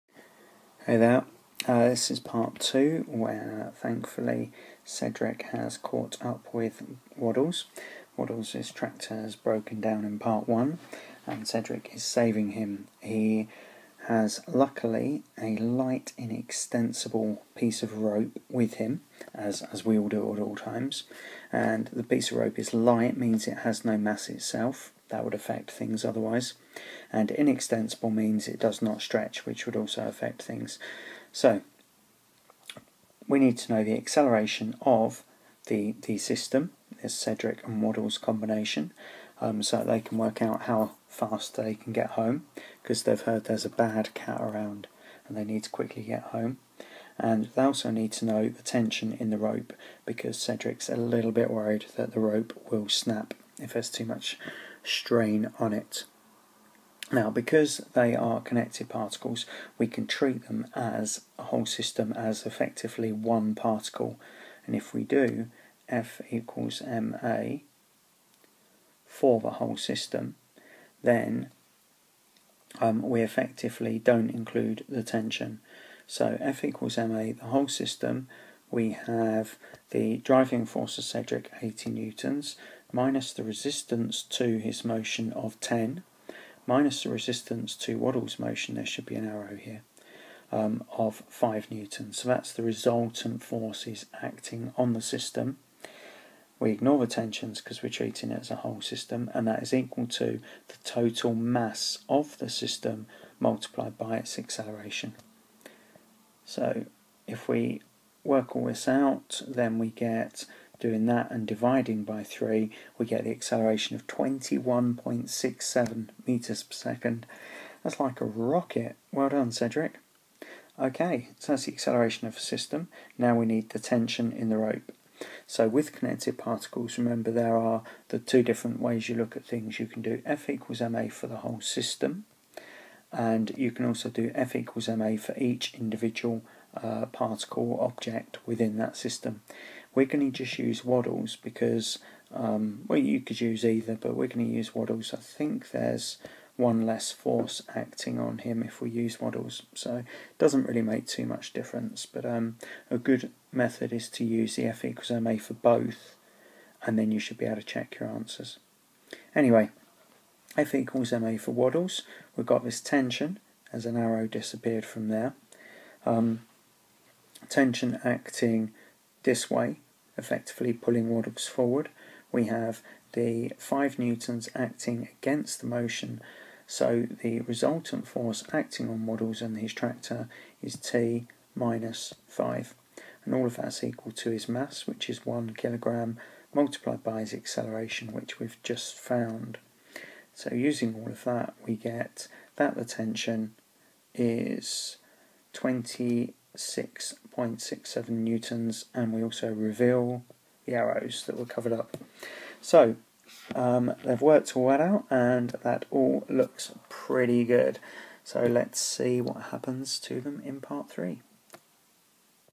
mechanics 1 - Connected particles 1
using newtons 2nd law to find the acceleration and tension in a connected particle situation